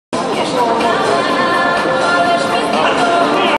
+90kg Meci09 Alexandru Marin VS Cristinel Gaju - Cupa Brăilei, editia I - 2005 - skandenberg
Cupa Brăilei, editia I - 2005 - skandenberg

skandenberg = armwrestling = arm wrestling